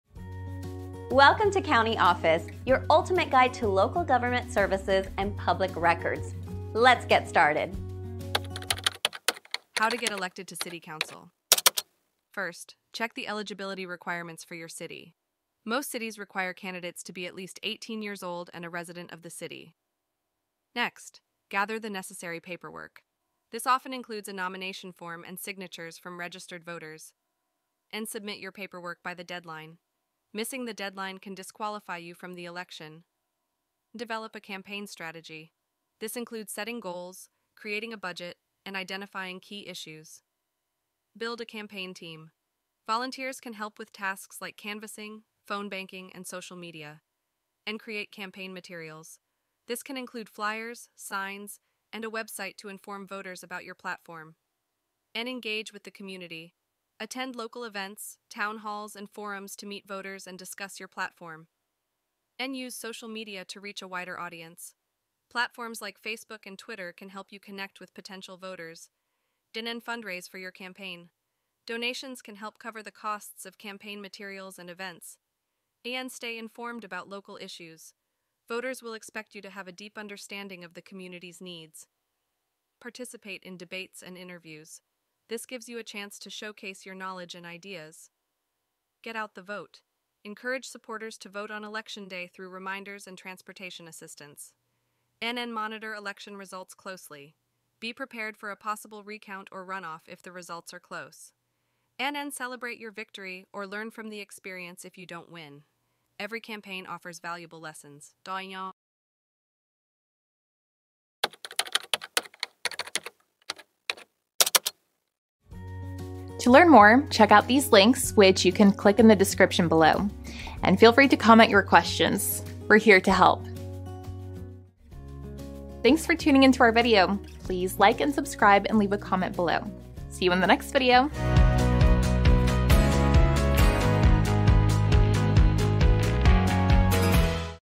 How To Get Elected To City Council? - CountyOffice.org
How To Get Elected To City Council? In this insightful video, we delve into the detailed process of how to get elected to City Council. From eligibility requirements to campaign strategies, we cover everything you need to know to embark on your political journey. Learn about the essential paperwork, campaign materials, and community engagement necessary for a successful campaign. Discover the importance of staying informed about local issues and engaging with voters through debates and social media. Whether you're considering running for City Council or simply curious about the process, this video offers valuable insights and practical tips to guide you through the election journey.